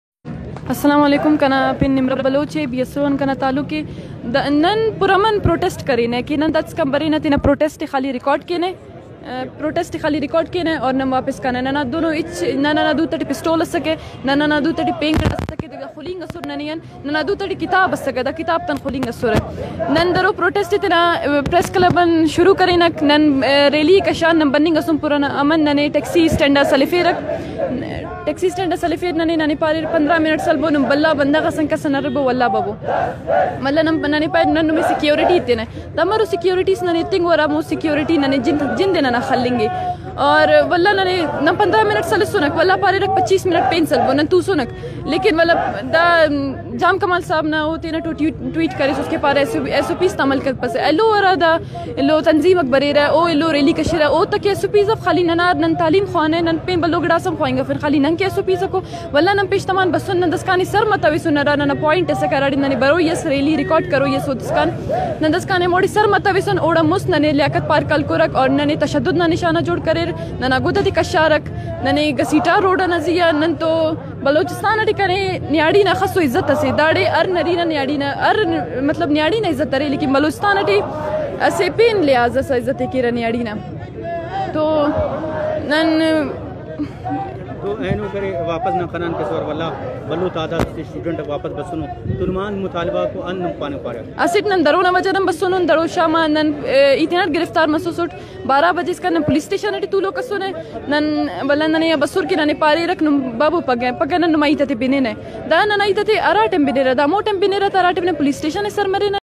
Nimra_rind_Baloch speaking to the media about online Classes. nimrabaloch. Dr-Mahrang-baloch
Nimra Baloch is a student activist of Baloch Students Organization.